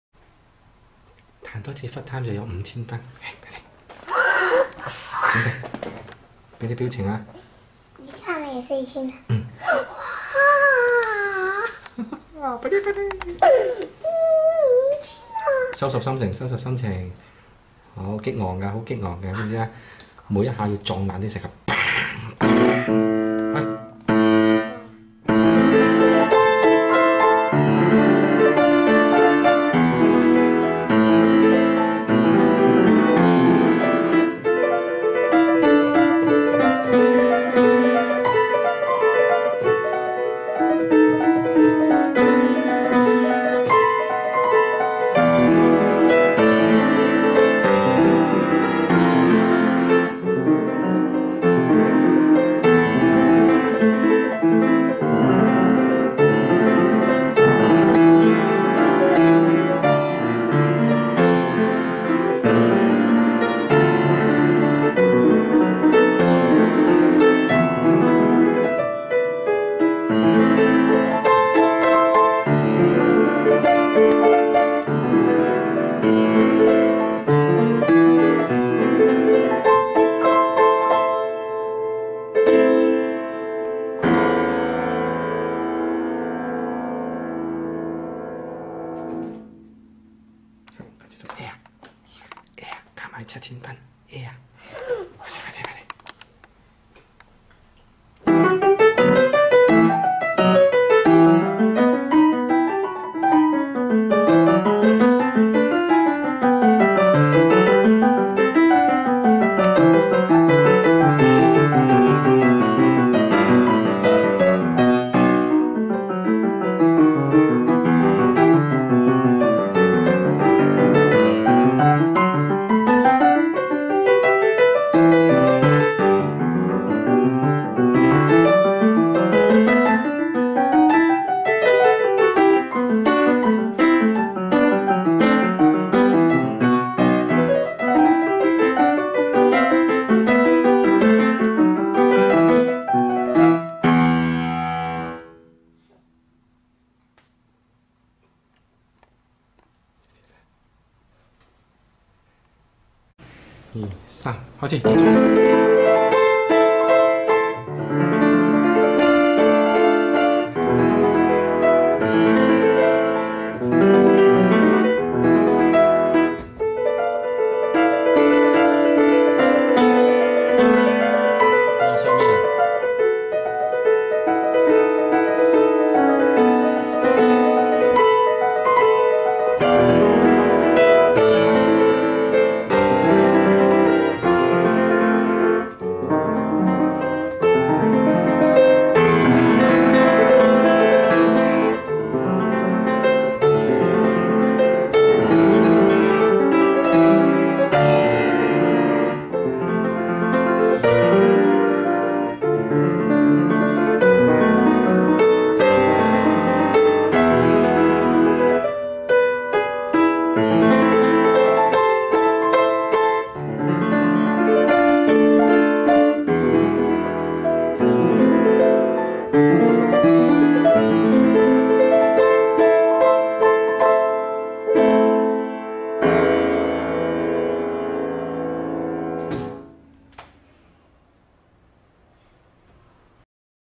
5 Yaşında Harika Piyano Çalan Çinli Çocuk - 5 Years Old Kid Plays Piano Incredible
5 Yaşında Büyük Yetenek - Big Talent At His 5 Years Old